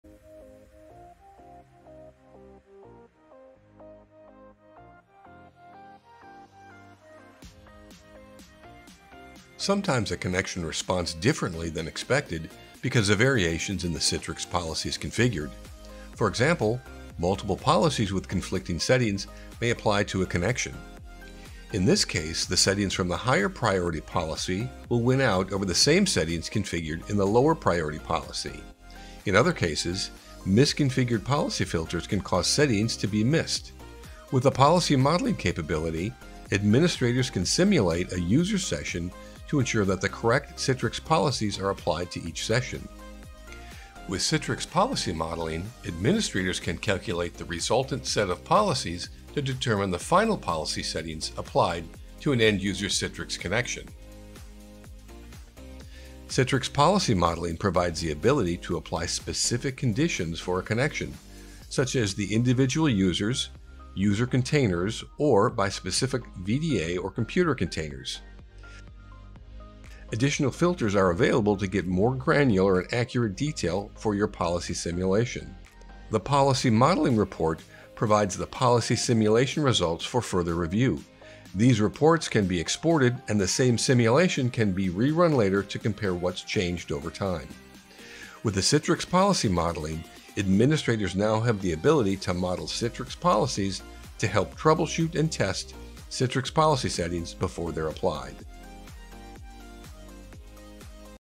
Citrix Features Explained: Citrix Policy Modeling
Citrix connections can sometimes respond differently than expected because of variations in the Citrix policies configured for your end user sessions. Using the Citrix Policy Modeling capability, administrators can simulate a user session to ensure that the correct Citrix policies are applied to each session.